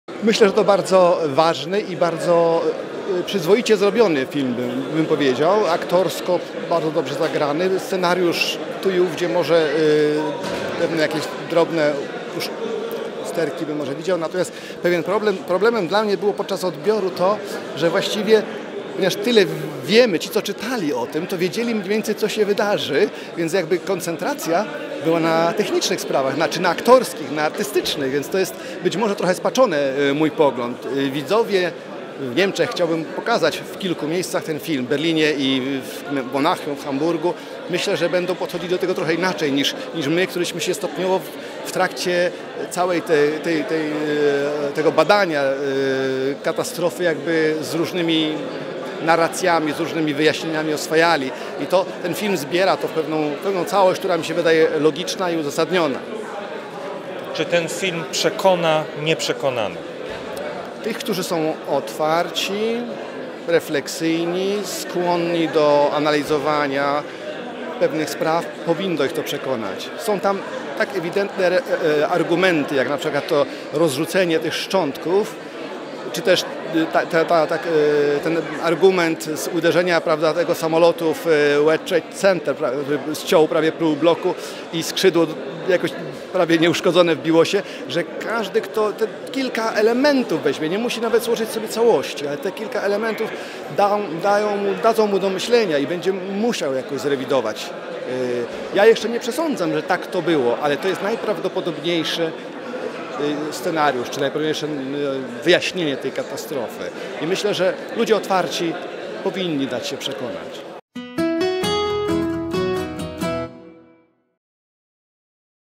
Ambasador RP w Berlinie o filmie "Smoleńsk"
Wypowiedź prof. Andrzeja Przyłębskiego - ambasadora RP w Berlinie - po projekcji filmu "Smoleńsk".